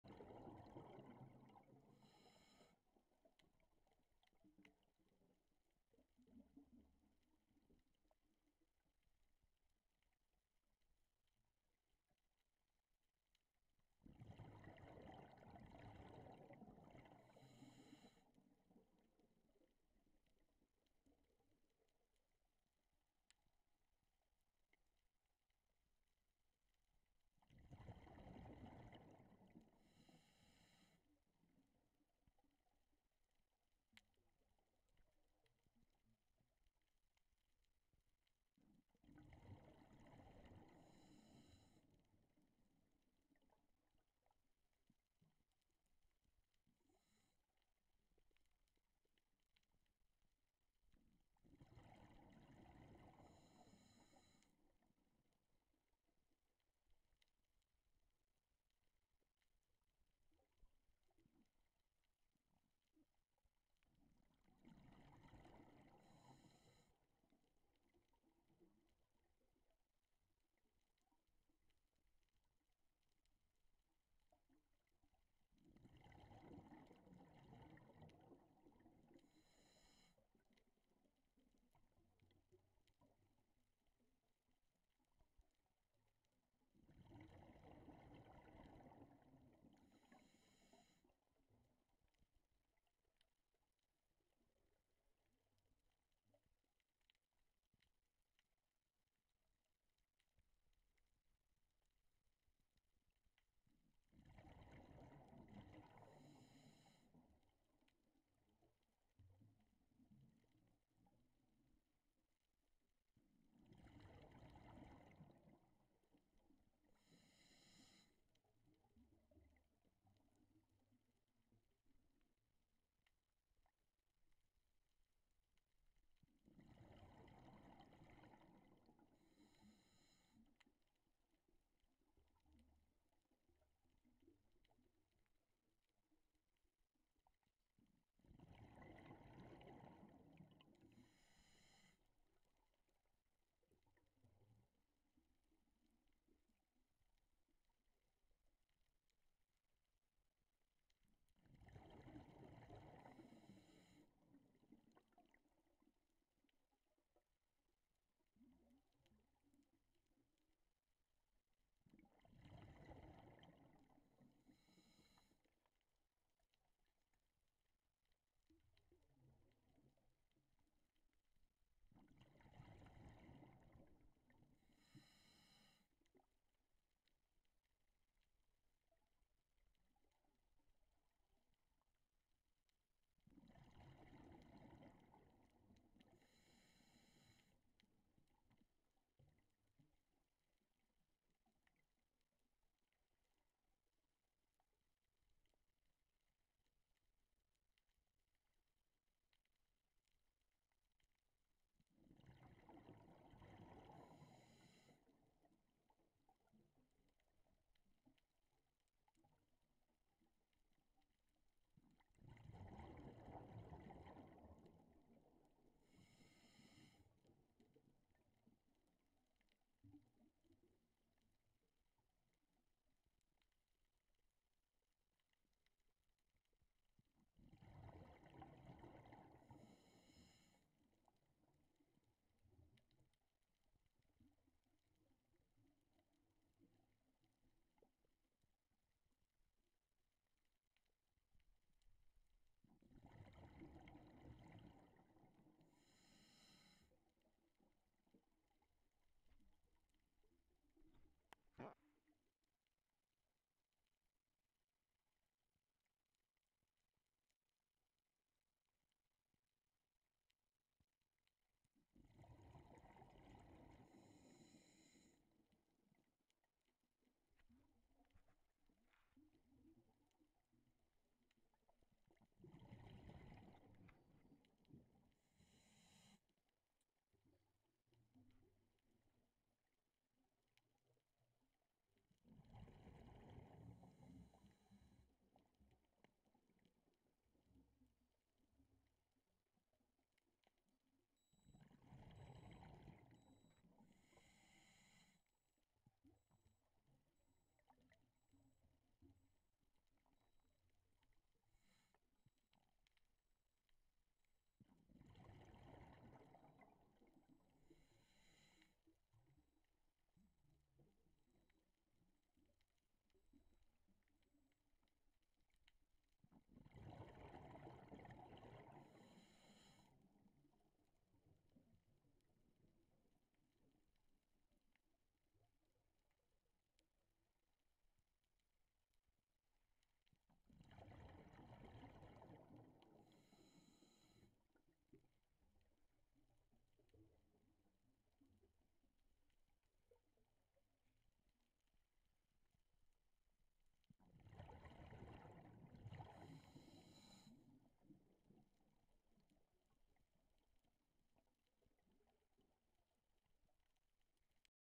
Salp Chain
Pyrosome 3-14-15 @ Junkyard aprox 33ft depth, Okinawa Japan.

A salp (plural salps) or salpa (plural salpae or salpas[1]) is a barrel-shaped, planktonic tunicate. It moves by contracting, thus pumping water through its gelatinous body. Salp jet propulsion is one of the most efficient in the animal kingdom.[2] The salp strains the pumped water through its internal feeding filters, feeding on phytoplankton.